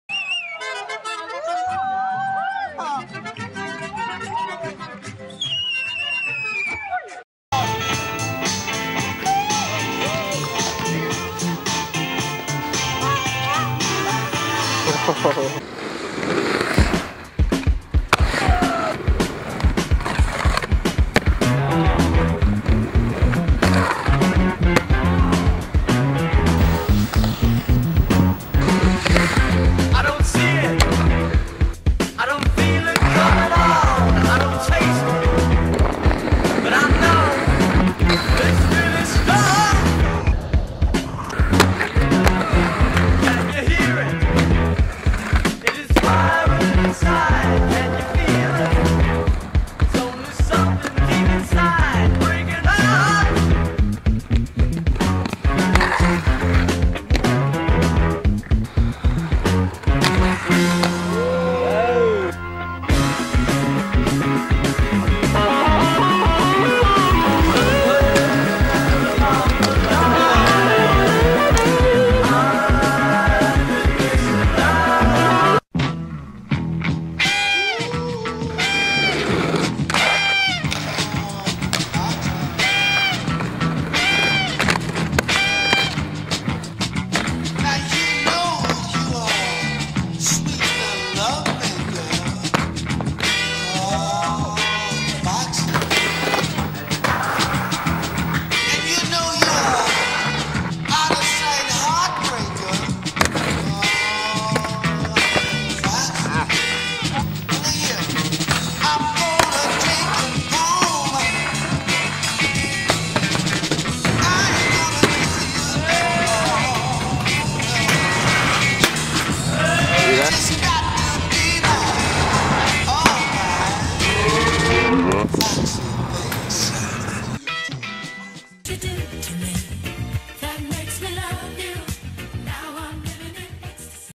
Dos Minutos #5
In this episode we highlight the talents of Razors team riders:
Derek Henderson & Joey Ihara.

To see more of these guys, go buy the Razors COTF video.